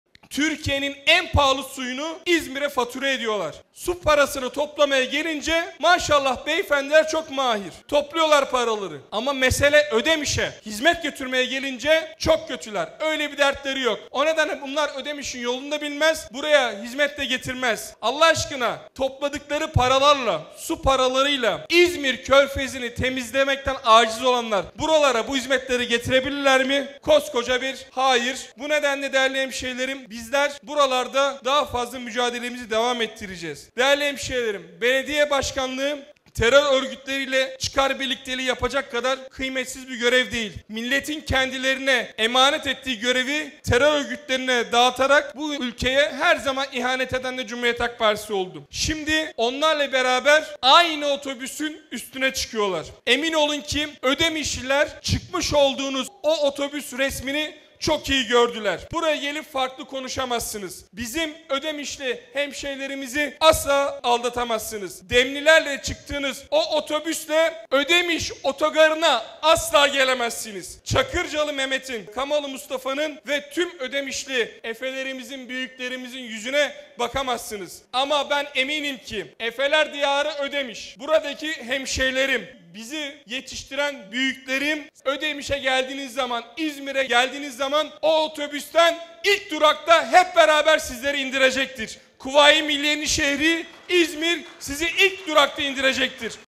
Türkiye’nin en pahalı suyunu İzmirliler kullanıyor ama İzmir Körfezi’ni temizlemekten acizler.
Türkiye’nin en pahalı suyunu İzmirlilere fatura etmekte çok mahirler ama İzmir Körfezi’ni temizlemekten acizler.

Bu anlayış İzmir’e tek bir kalem icraat getirebilir mi?